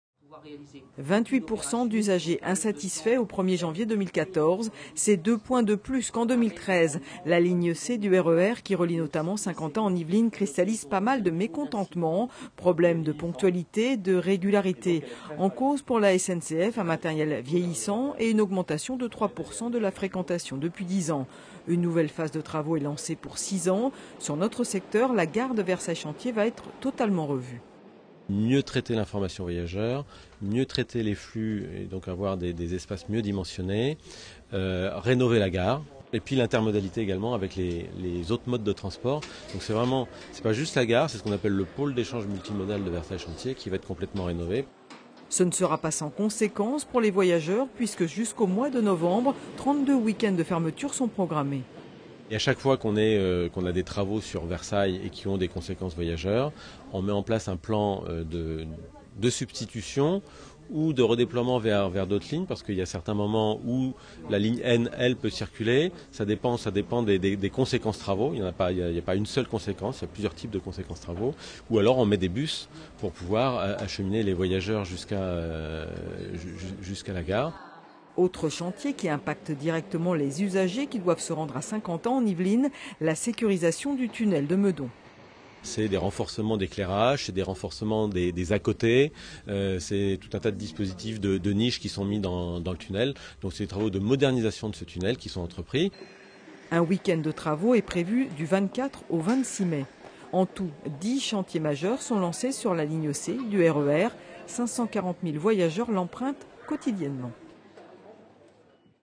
150 millions d'euros pour la rénovation de la ligne C du RER
En 2014, 72% des voyageurs de la ligne C du RER étaient satisfaits. Ils étaient 74% en 2013. Problème de régularité, de ponctualité : les griefs s'accumulent contre une ligne qui connait une augmentation de sa fréquentation de 3% par an. Pour améliorer ses résultats, la SNCF lance dix chantiers sur une période de 6 ans, pour un montant total de 150 millions d'euros.